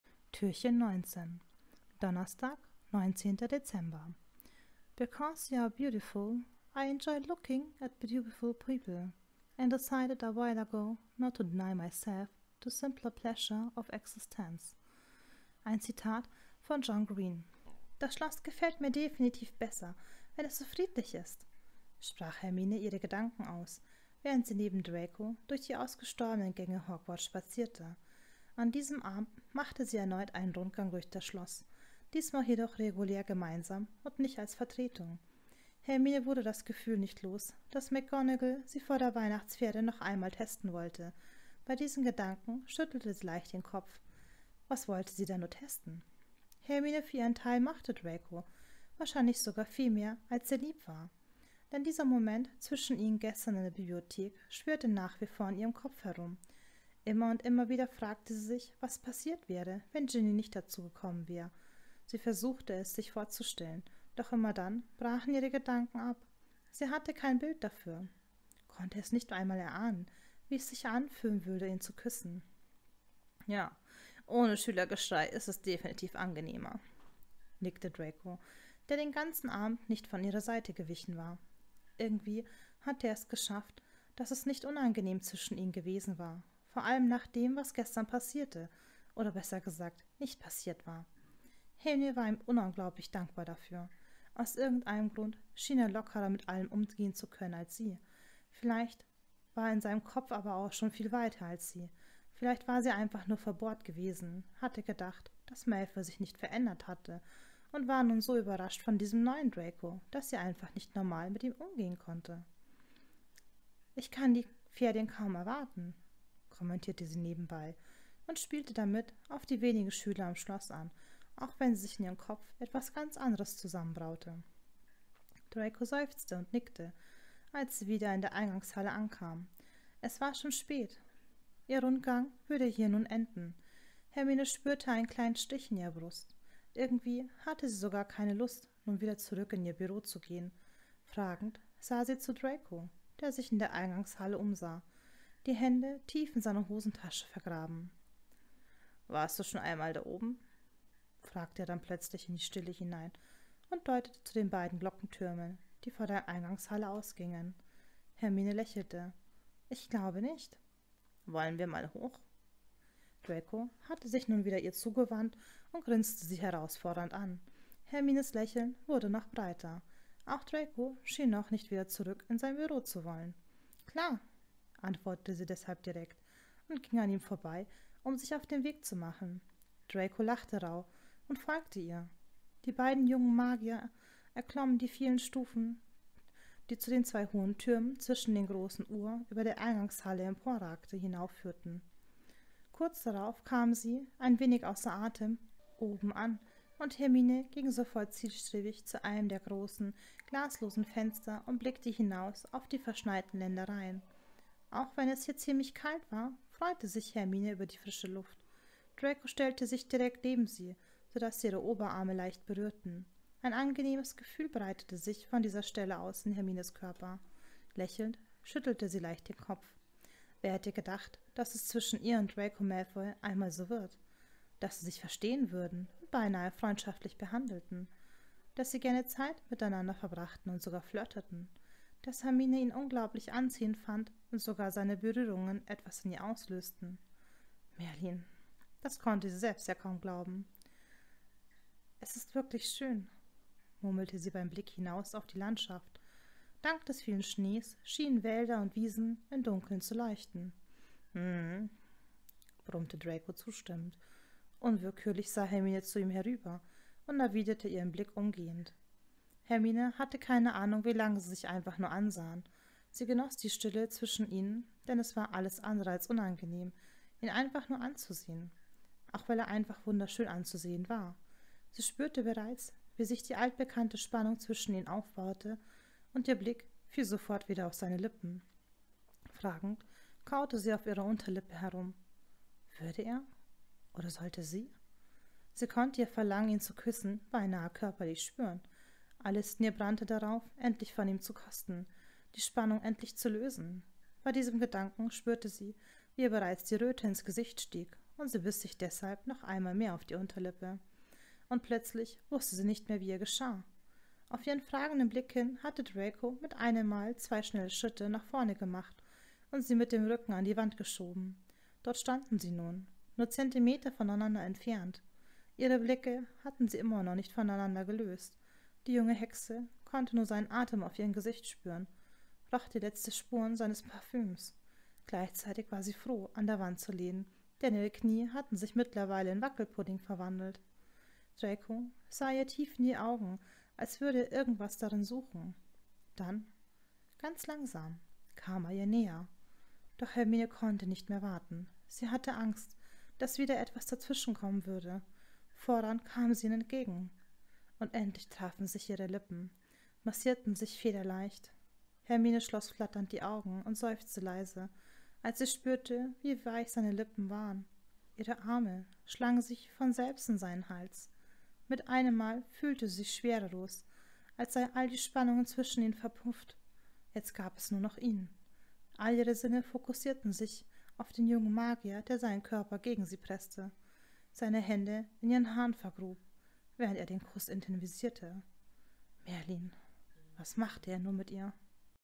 Dramione Adventskalender 2020 ~ Winter nights: A christmas story - Türchen 19
Adventskalender Speical 2020
Dramione Fanfiktion
Zeit-Geist - Winter nights: A christmas story

Kurzbeschreibung:

„Gemeinsam mit Ginny und Neville beobachtete Hermine, wie der Nachzügler McGonagall entschuldigend zunickte und sich dann ebenfalls einen Platz in der letzten Reihe suchte – auch wenn er gar nicht so aussah, als täte ihm irgendwas leid. Eigentlich sah er genauso aus, wie Hermine sich gerade fühlte. Kein Wunder, sie konnte sich auch nicht vorstellen, dass Draco Malfoy sich für Weihnachten begeistern könnte.“ ~ Weihnachten in Hogwarts. Das bedeutet Kitsch, Festessen und ganz viel Liebe. Absolut nichts für Hermine Granger, Lehrerin für Verwandlung. Sie hält nichts von Romantik. Und genauso wenig hält sie von ihrem Kollegen Draco Malfoy. Das dachte sie zumindest vor der Weihnachtszeit.